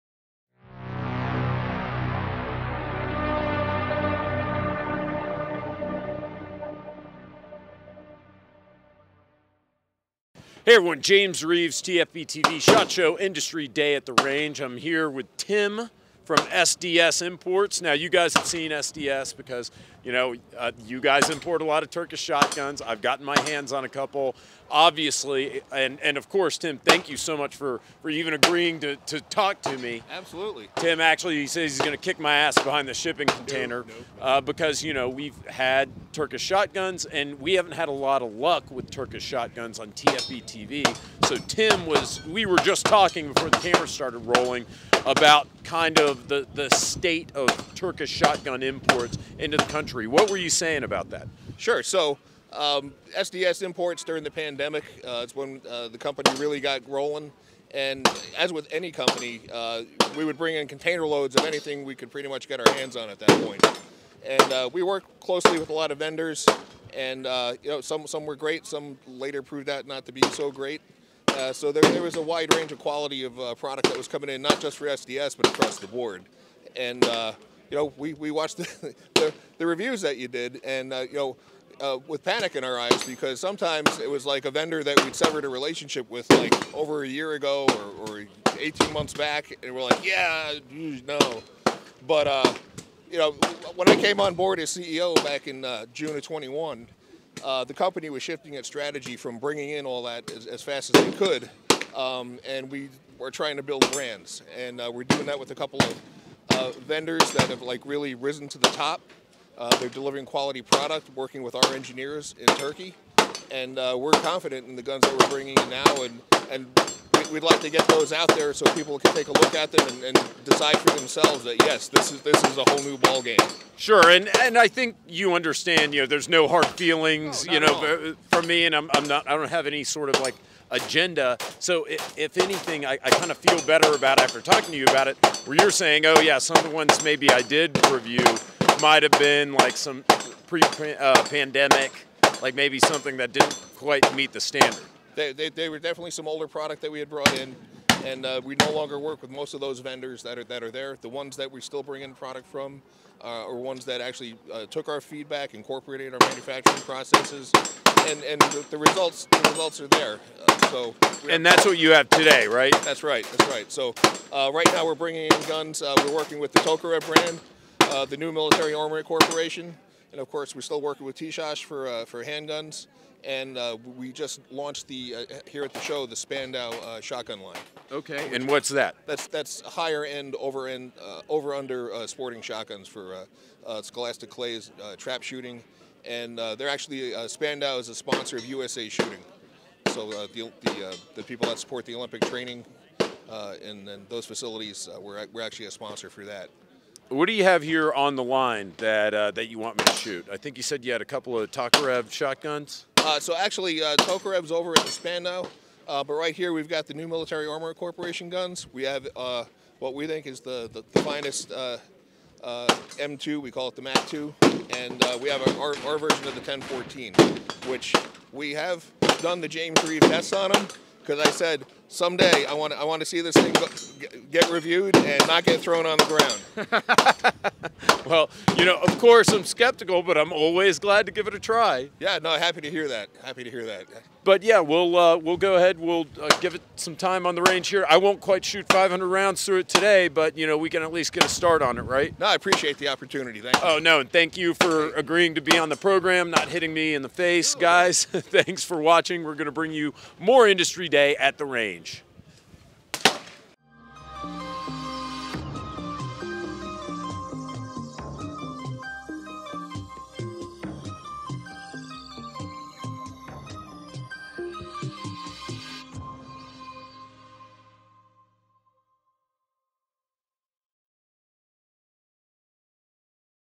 A Turkish Shotgun That Doesn't Suck at SHOT 2023?
In this episode of TFBTV, SDS boldly challenges @JamesReeves, slayer of Turkish shotguns, to try the new Military Armament Corporation M2 shotgun. James gets his hand on this new Benelli M2-style shotgun for a brief moment at the range, but will it stand up to the 500 round torture test? Time will tell.